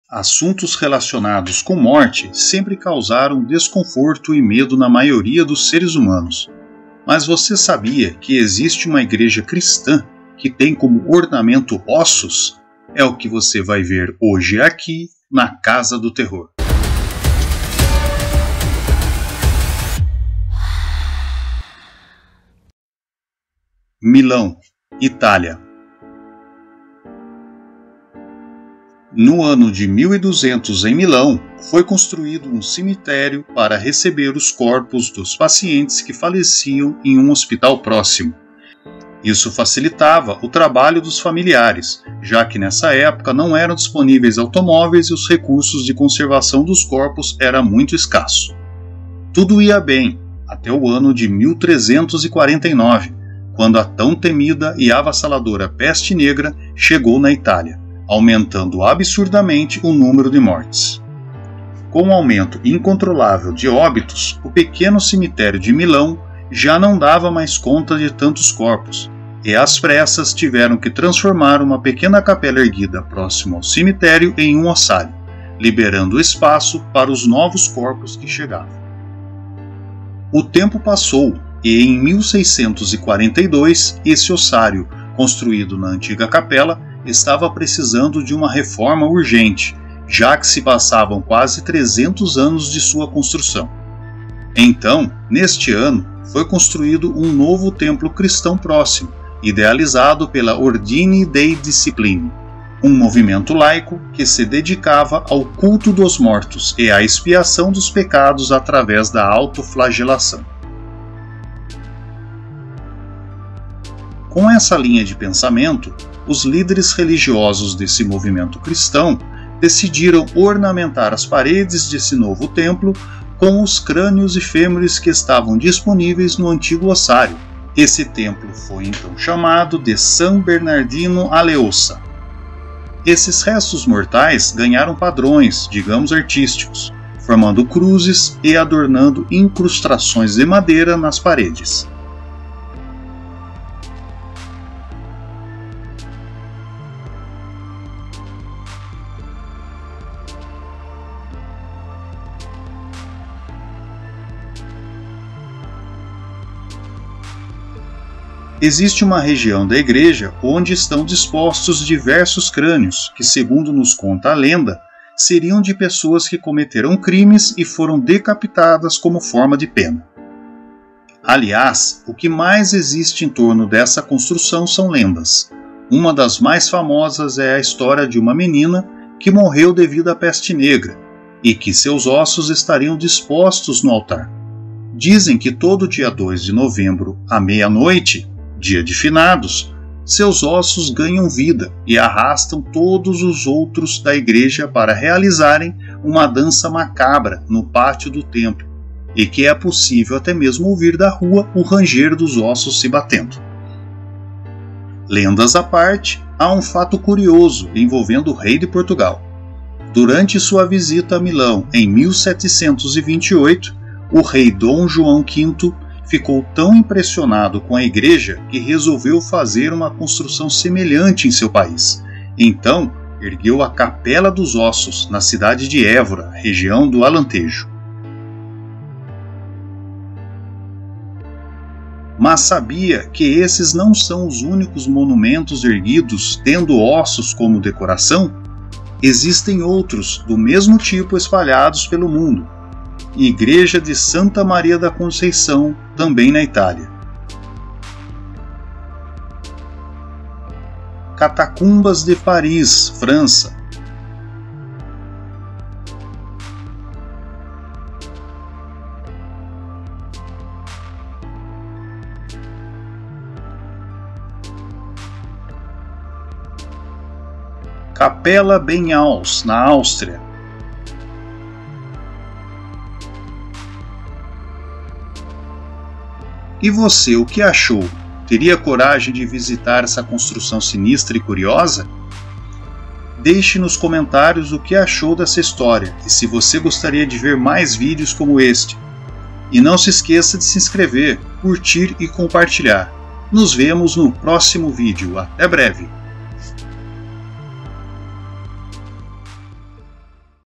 Conheça a Igreja de Ossos de Milão
Neste vídeo vamos conhecer a história por trás da igreja de Ossos de Milão na Itália, e porque ela tem ossos nas paredes. Também conheceremos outras igrejas pelo mundo que também possuem essa sinistra decoração com ossos.